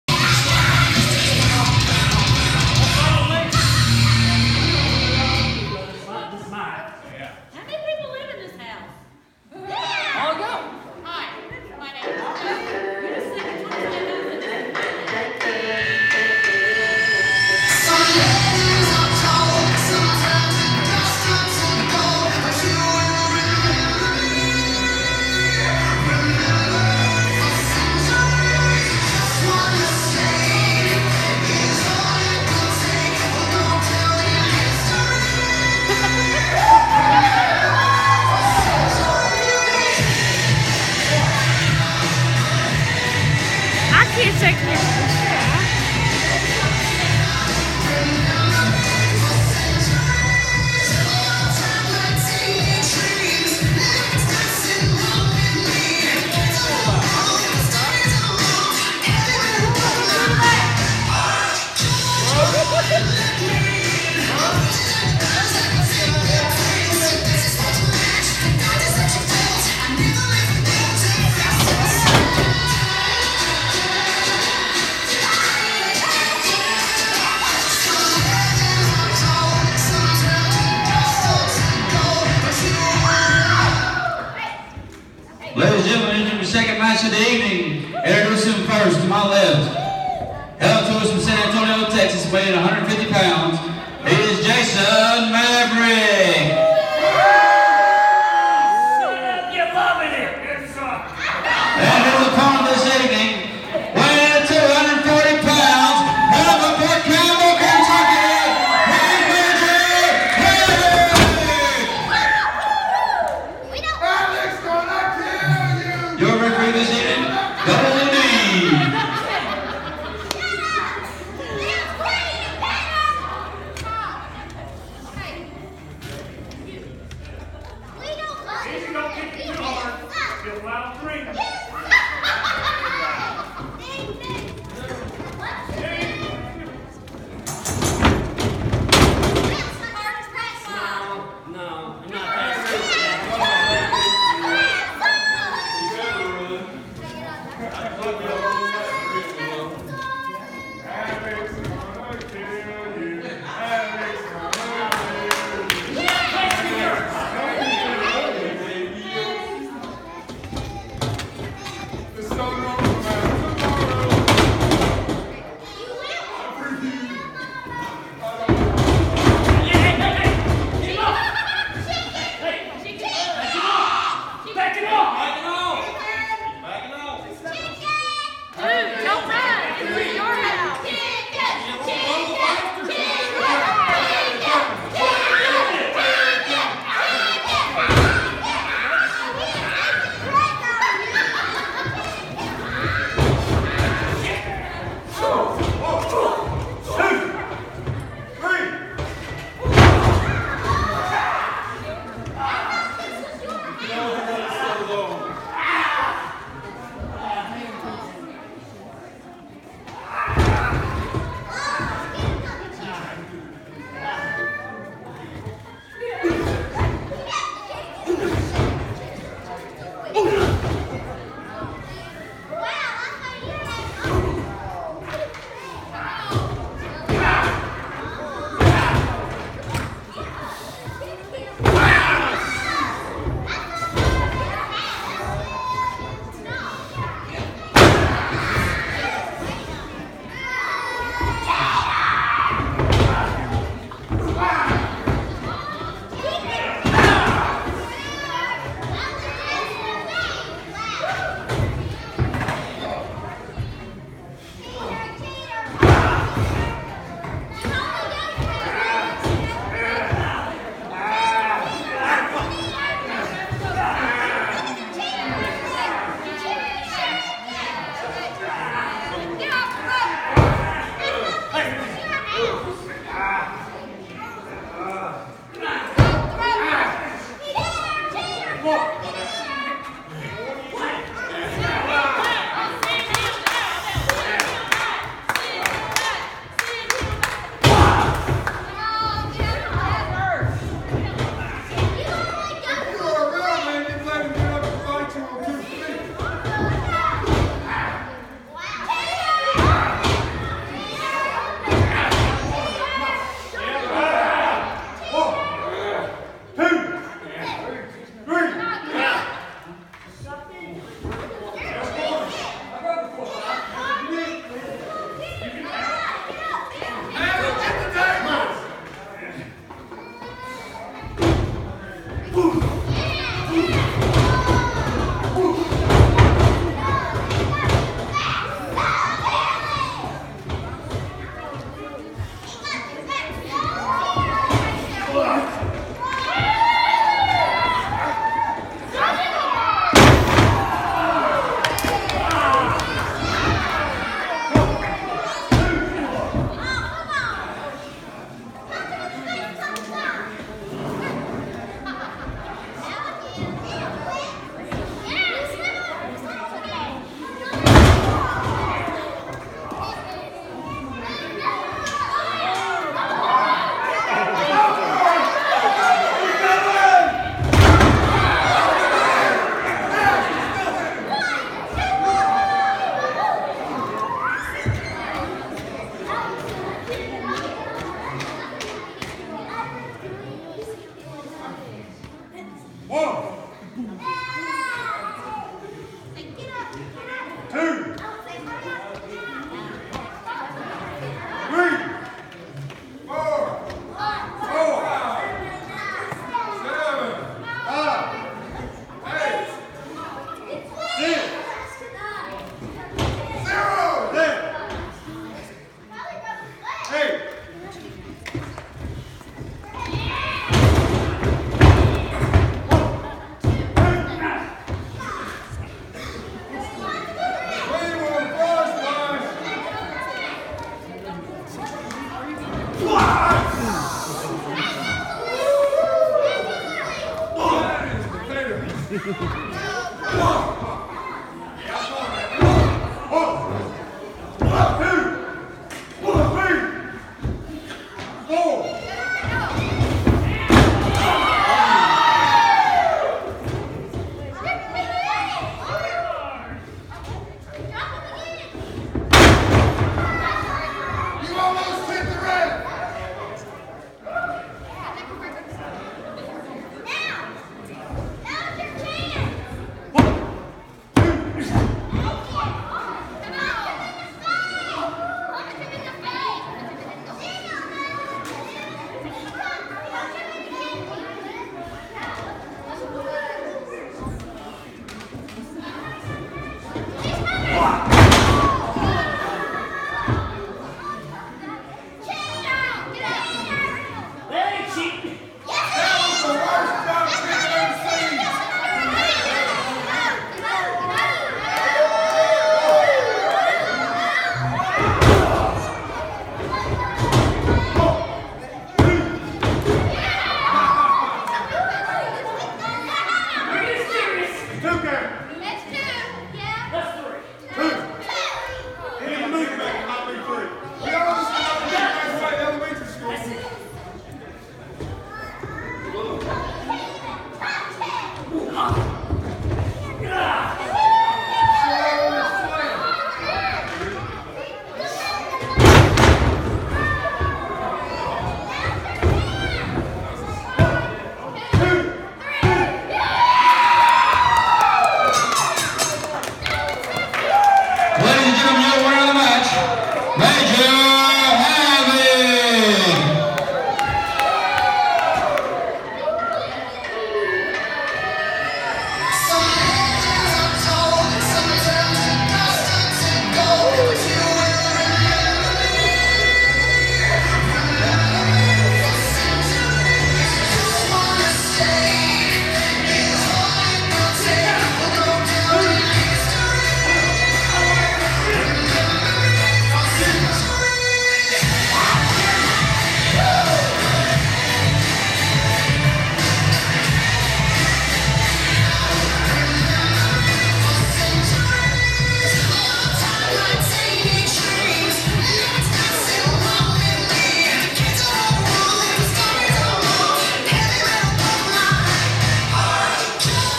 WASP Jason Michaels VS Major Havoc
The Maverick Jason Michaels and Major Havoc face off for the first time.